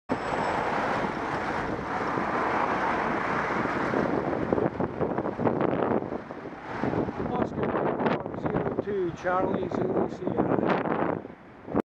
UO02CZS drives through red light while on the phone
Camera can't see the phone because he's pressing it to his left ear with his right hand, but I could see it.

Friday 23rd January 2015 approx 17:20